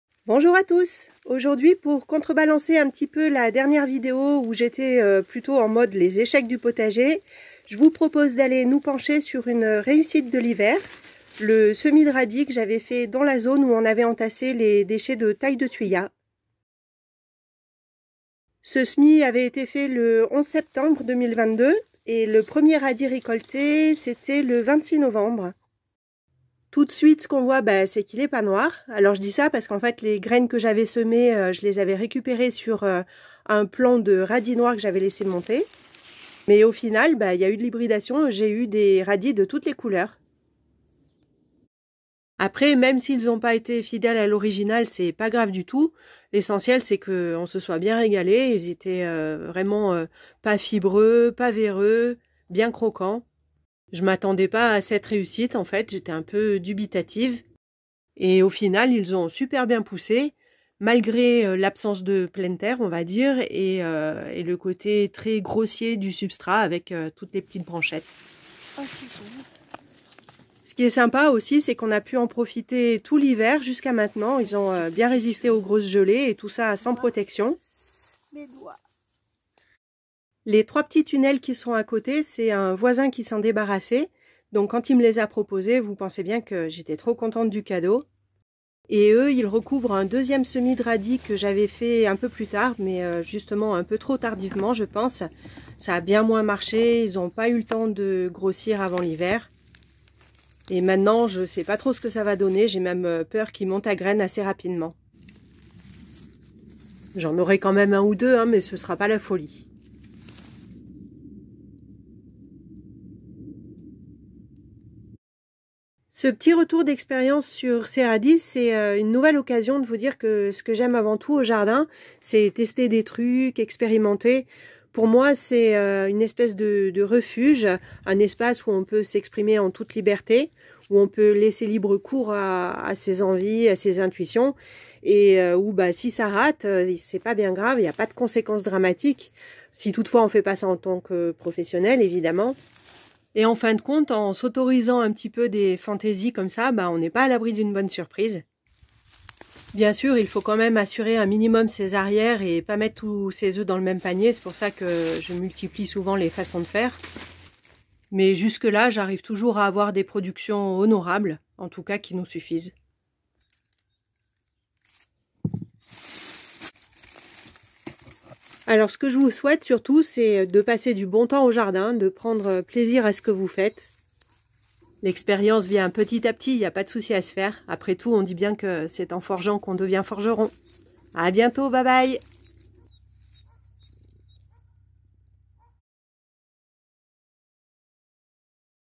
Radis d'hiver dans le thuya : ça marche !
La finalisation du montage de cette vidéo a été un peu longue... à l'heure où je mets en ligne, mon cœur est attristé par la perte d'un être cher. Malgré tout la saison potagère recommence, et ça c'est une bonne nouvelle 😉 On recommence à semer des radis de printemps, alors zoom sur ceux qui nous ont régalé cet hiver.
A très bientôt, bon jardinage !